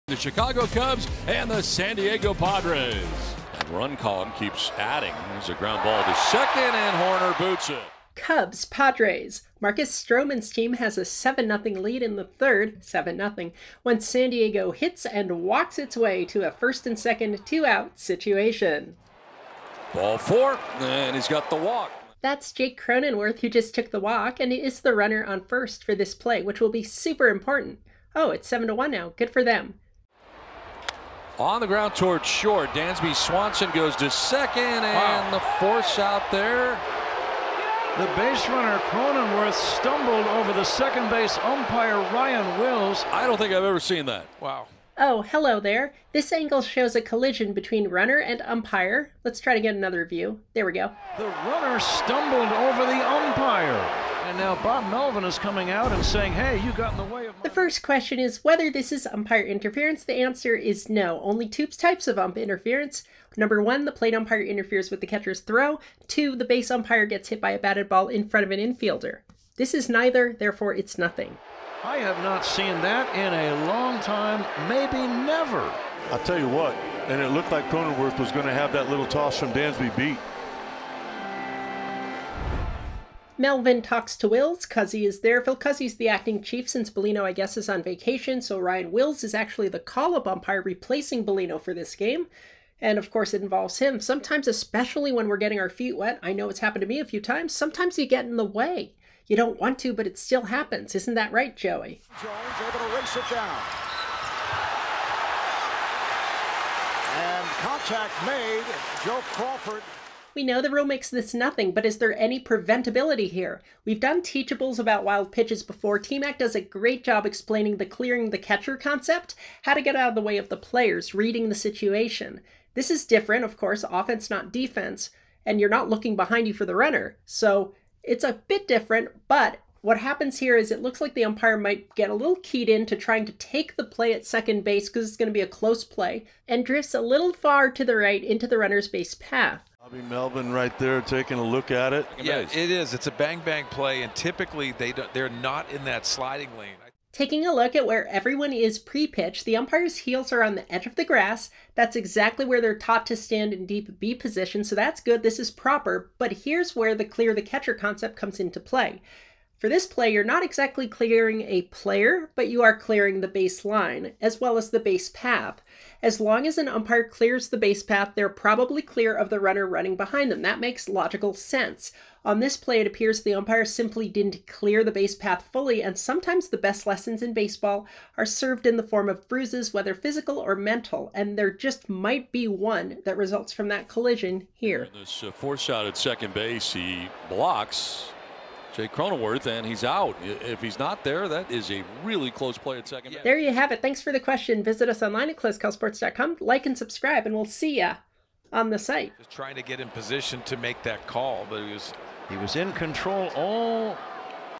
Jake Cronenworth Runs Into Umpire, Ruled Out at 2nd Base - Was it Interference?
Padres baserunner Jake Cronenworth ran into 2B Umpire Ryan Wills on an infield ground ball as he tried to reach second base, falling over and allowing the Cubs plenty of time to get the out. Should this have been umpire interference or is this nothing...and where should an umpire stand on a play at second base? Article

This is not umpire interference since UMP INT only has two types
1) Umpire interferes with catcher's throw to retire runner or return ball to pitcher
2) Umpire hit by BATTED BALL inside infield before it passes an infielder, provided it did not first touch the pitcher.

We also talk about umpire positioning and how clearing the catcher—a concept used for umpiring wild pitches—can be modified for use here where a 2B Umpire working inside from Deep B can clear the baseline and potential base path of the runner to avoid the likelihood of a collision from the runner in back of them.